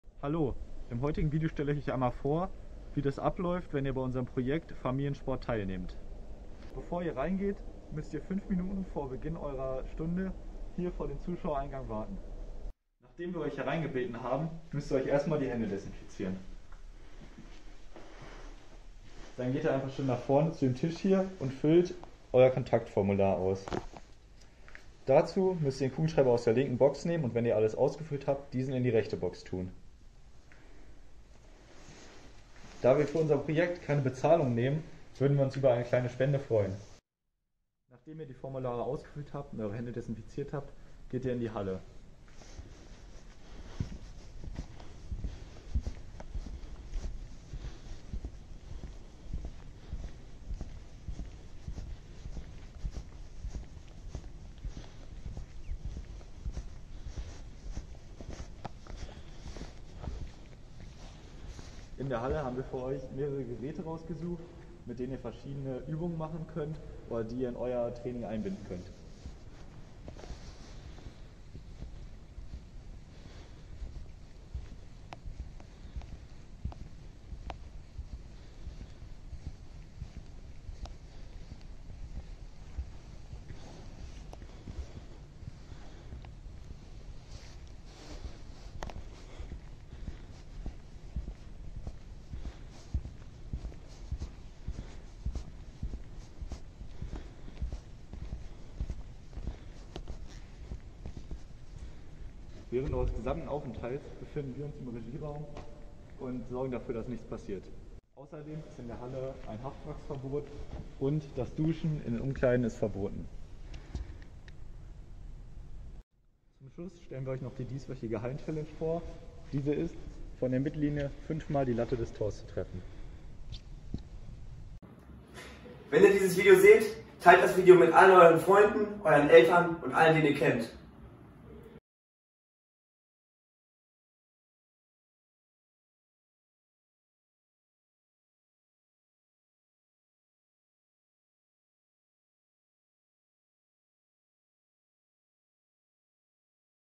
Projekt Familiensport
In diesem Video erklären wir euch den Ablauf an der Halle wenn ihr euch beim Projekt Familiensport auf unserer Website angemeldet habt.